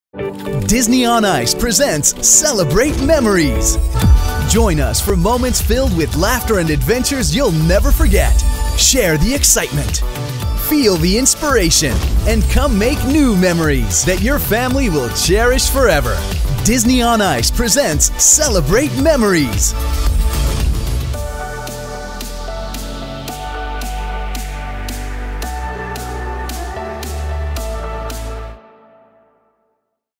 Disney On Ice Presents Celebrate Memories
Disney On Ice Presents Celebrate Memories is coming to Van Andel Arena with 8 shows February 27 - March 1, 2020.